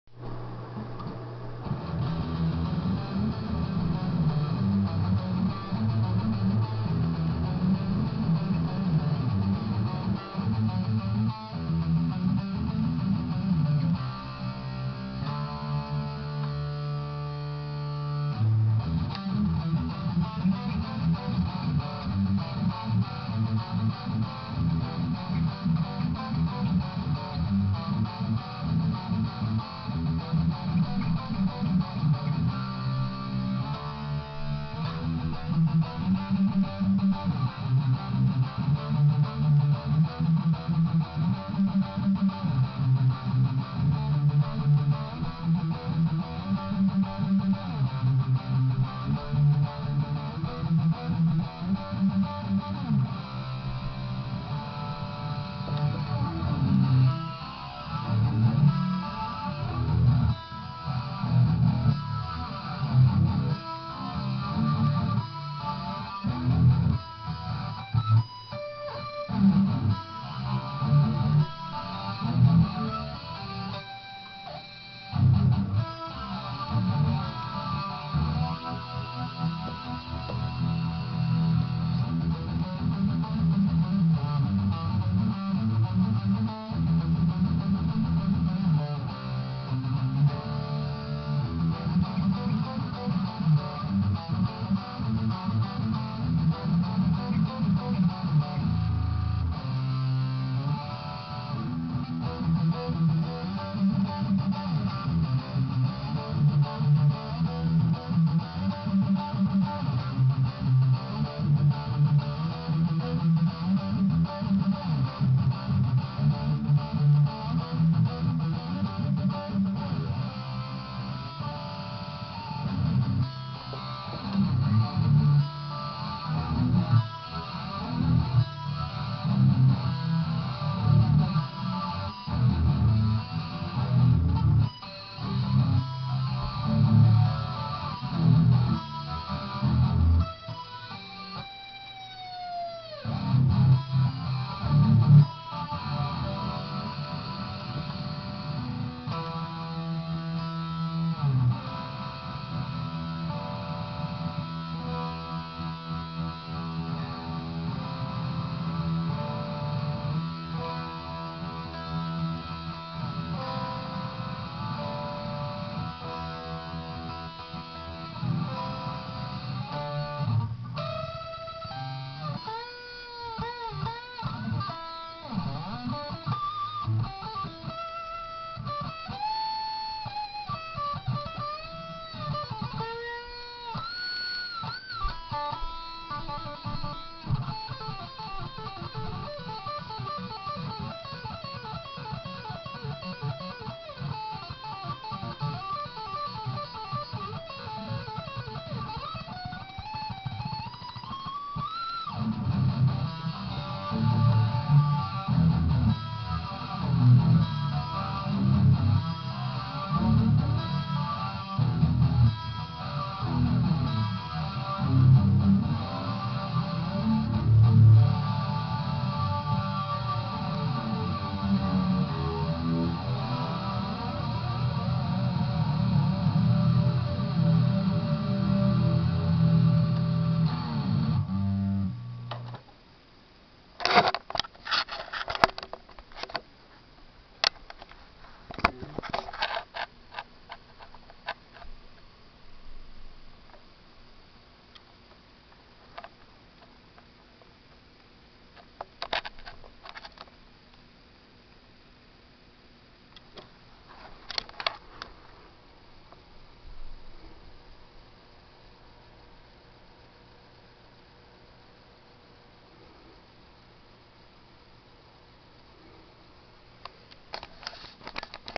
the greatest song ever it will change the world
guitar to wah(unused)to grunge to chorus to flange to amp
the sound is messed uo, so turn the volume up
ok im playing both guitar parts cuz the other guitarist isnt here so i made a few mistakes.
Fab Flange-Regen 8 o clock
speed- 6 oclock
depth-all up
chorus
Mix- 9 oclock
speed 6- oclock
depth-6 oclock
grunge
loud 11 oclock
low 3 oclock
high- noon
grunge- all the way up

crate gx 212 amp clean chanel
volume-.5
low 7
 mid 6
 high 7